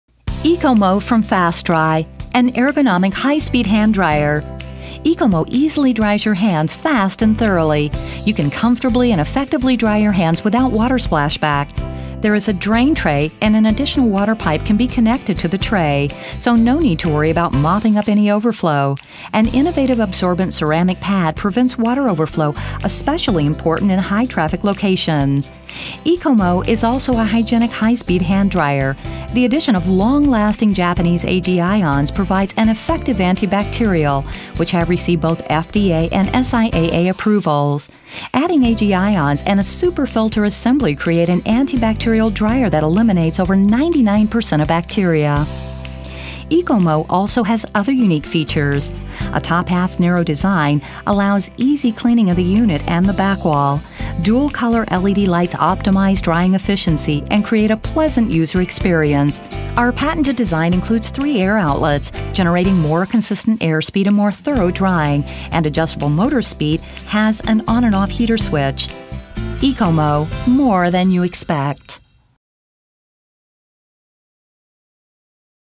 EcoMo: a hygienic, high-speed hand dryer for the human form
Why EcoMo?

1. A good innovation always begins with a human requirement.
Comfortable drying with no physical comparison.
2. Nature Drying Position: No Back Pain.
3. Wheel-Chair Friendly.
4.Super HEPA Filter-Eliminate over 99% of current bacteria. No fear of bacteria.
5. Absorbent Ceramic Pad: Prevent water overflow.
6. Good air ventilation and easy to clean: No bad odors or trash left inside.
7. Patented air outlets: Prevents water from splashing back on users.